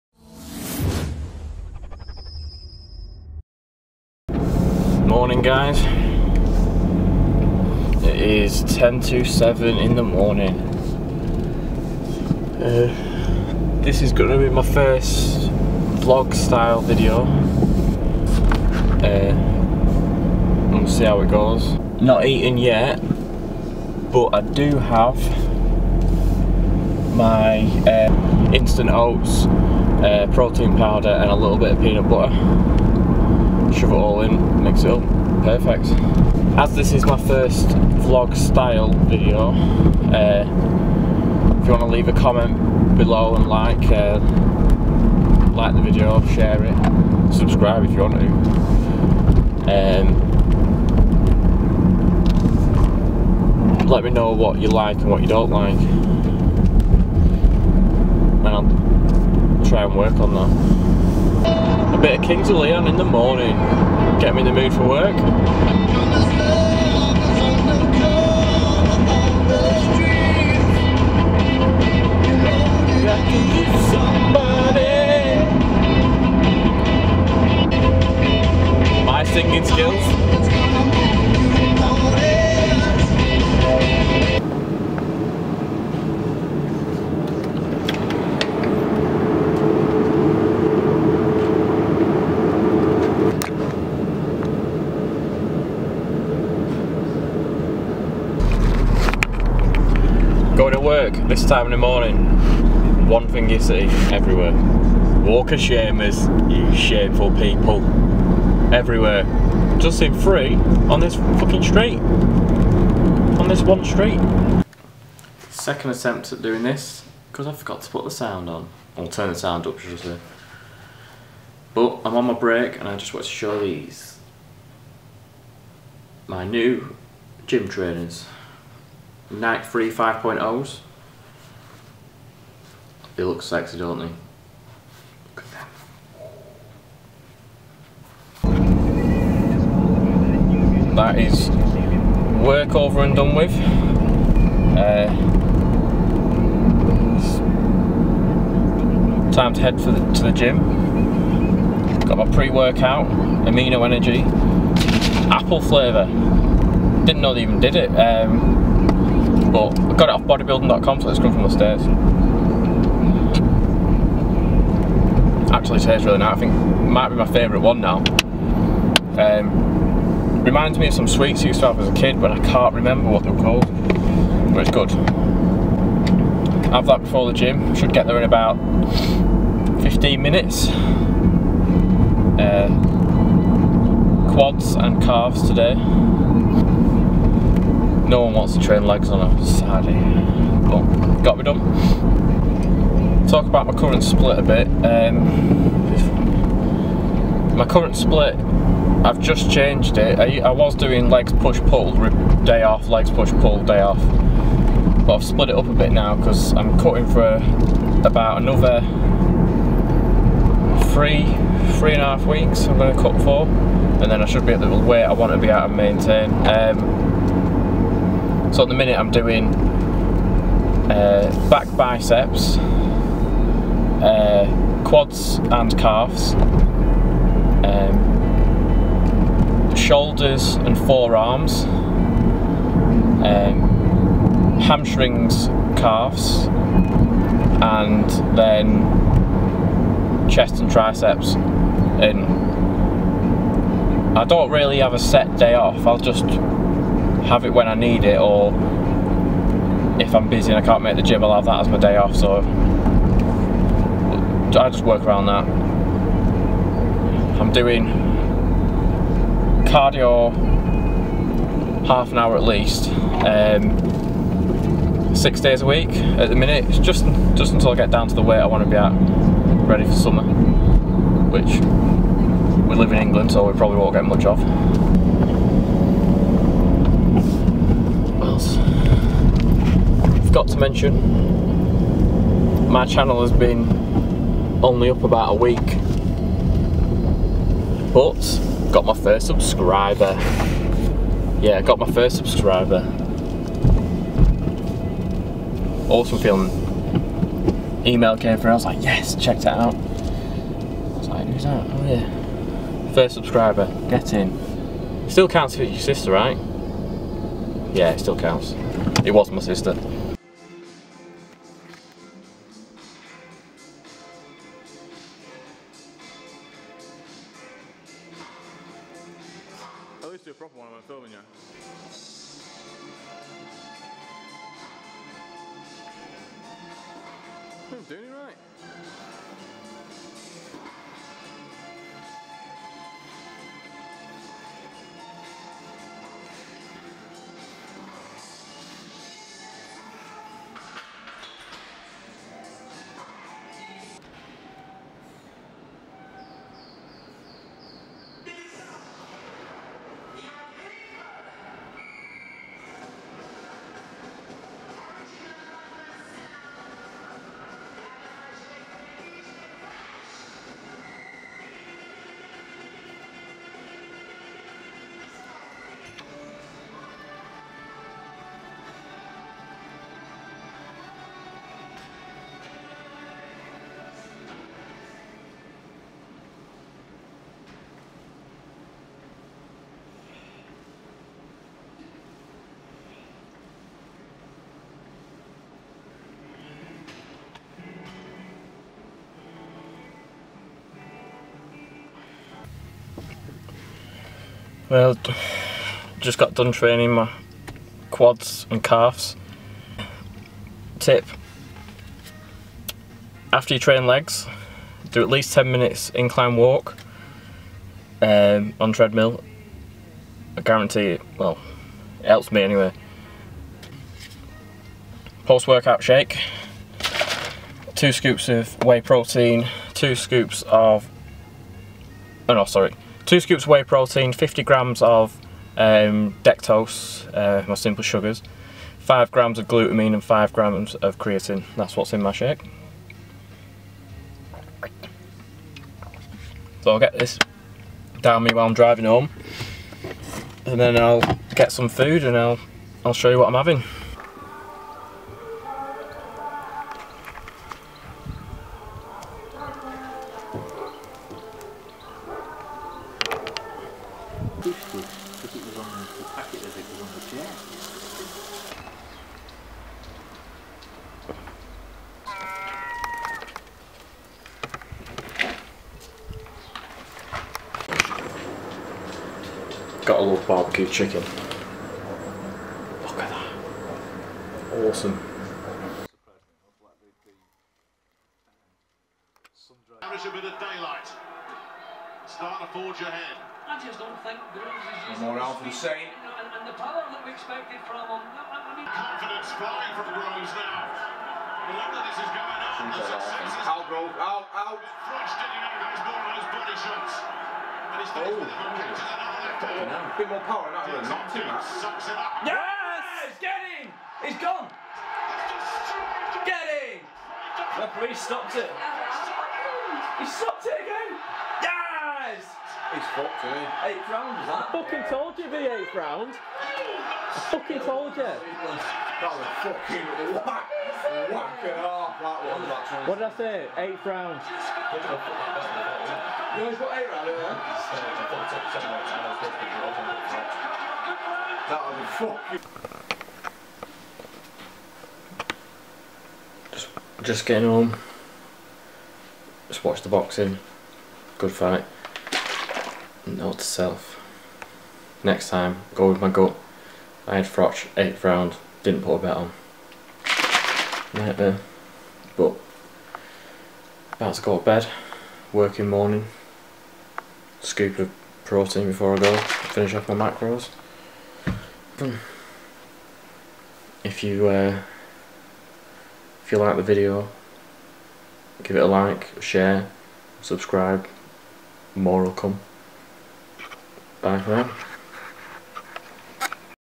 VLOG Session#1 - Quads Calfs, BBQ & Boxing
My First VLOG Session - New Trainers, Training, My Split, Having BBQ and watching the boxing!

***FOLLOW ME***